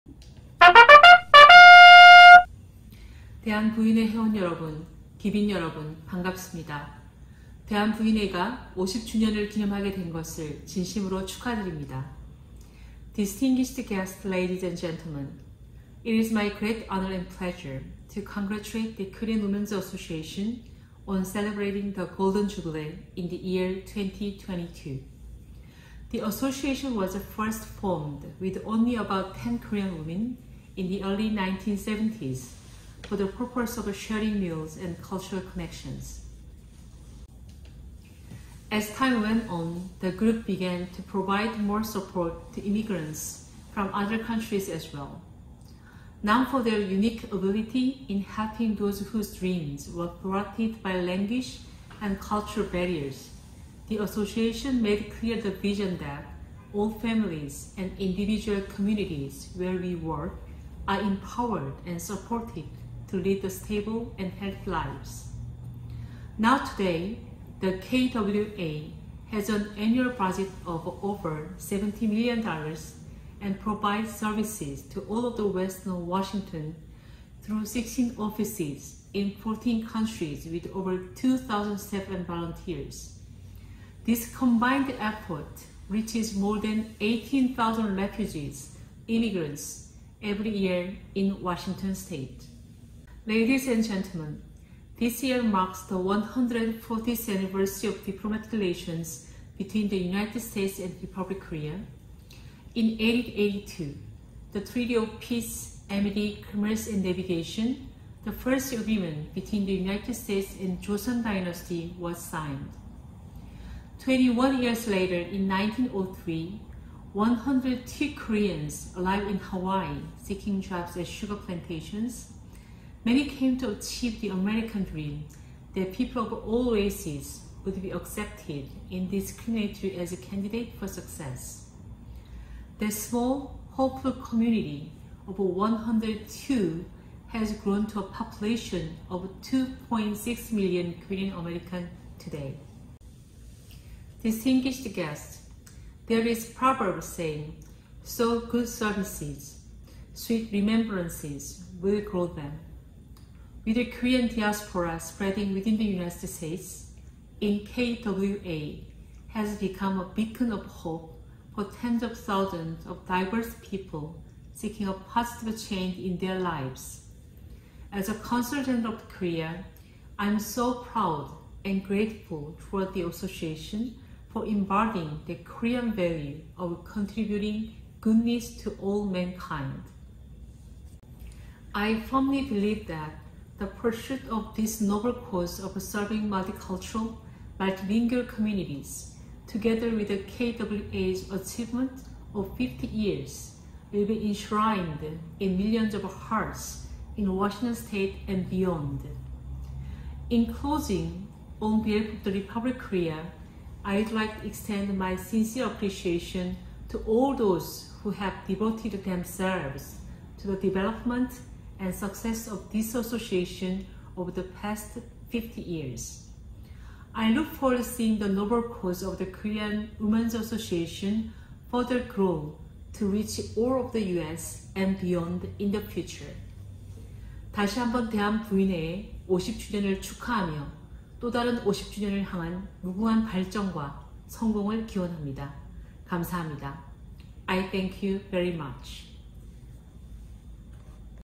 Celebrating the Golden Jubilee of the Korean Women’s Association (KWA) 대한부인회 50주념 기념 서은지 총영사 축사
CG Seo congratulate the Korean Women’s Association (KWA) on celebrating the Golden Jubilee in the year 2022.

서은지 총영사는 대한부인회 50주년 행사를 축하하였습니다.